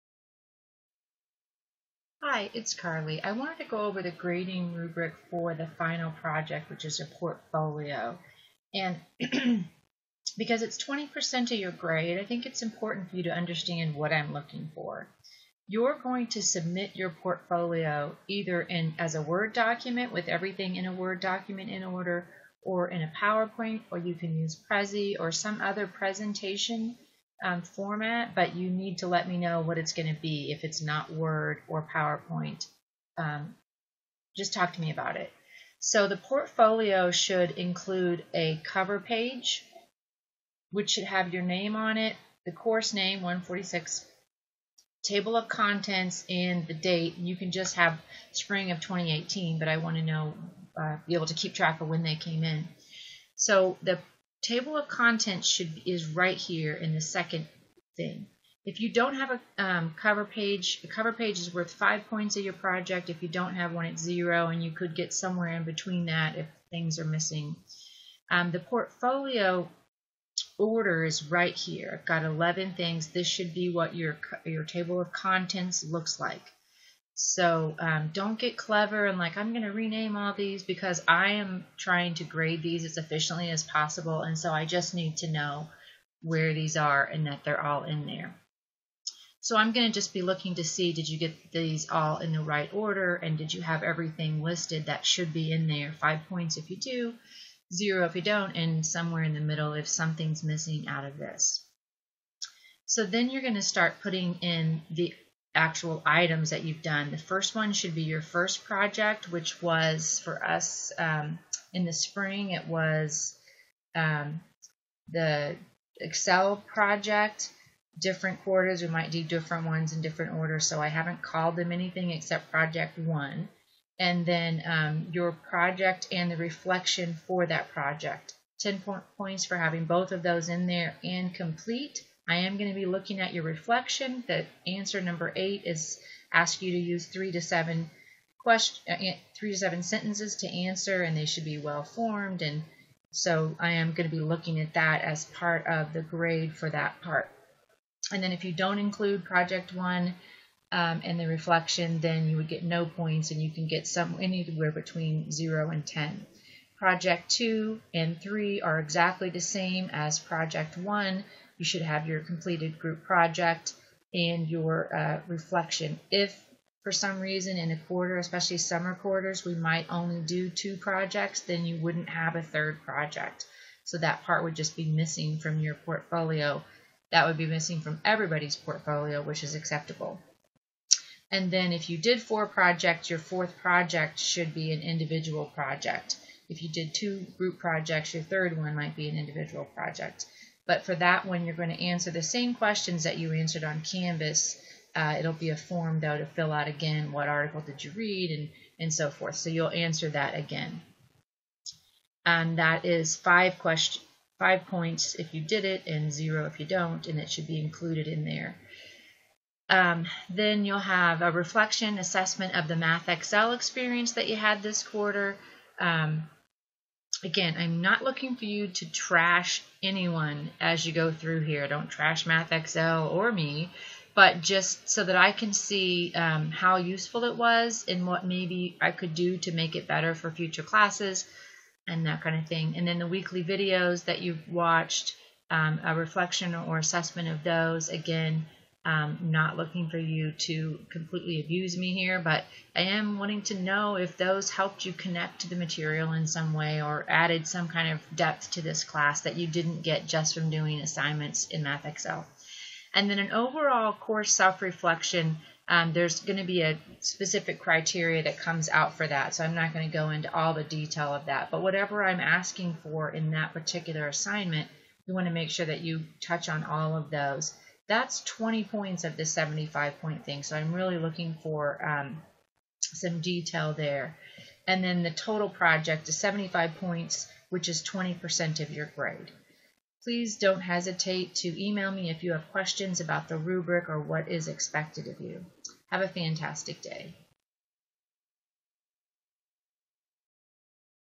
Statistics: Final project rubric
This video explains the grading rubric for the final portfolio for math 146.  Please note, not every quarter will have the same number of projects.  Include all that you have completed.